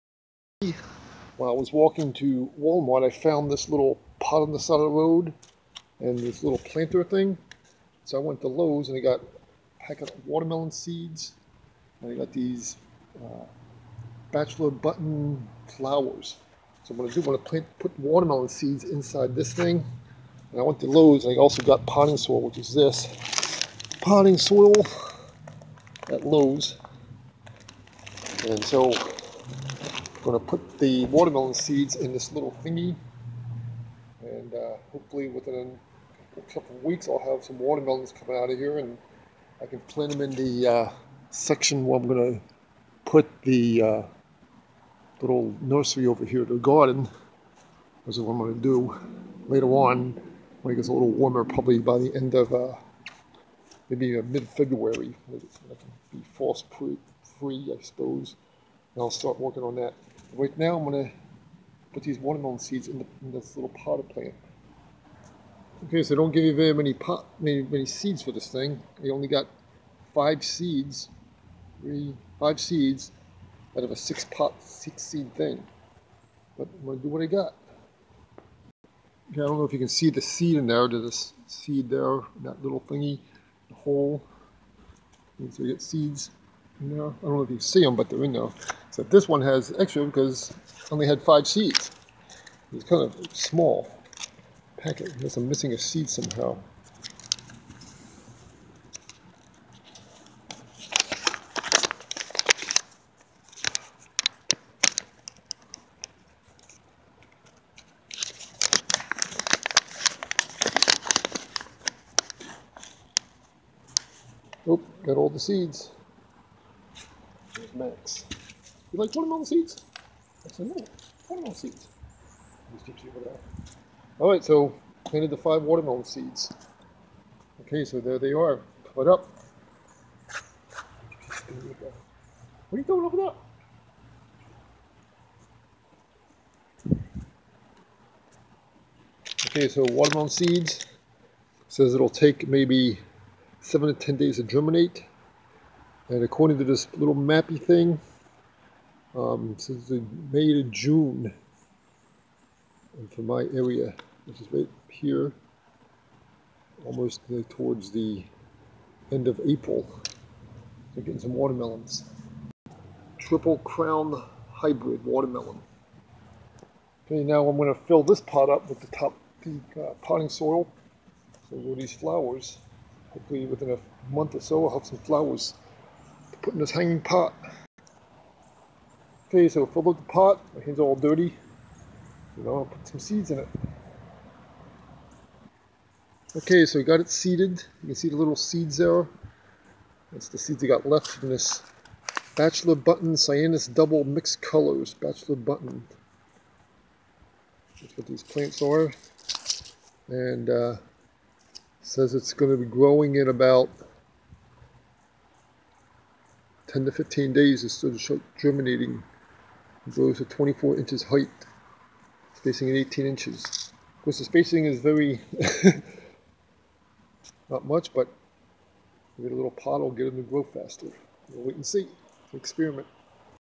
Watermelon and Flower Seed Planting.
Found a Seed Plant while walking to Wal-Mart.  I then got some Potting Soil and some Seeds to plant Watermelon and Flower Seeds.  Triple Crown Hybrid Watermelon.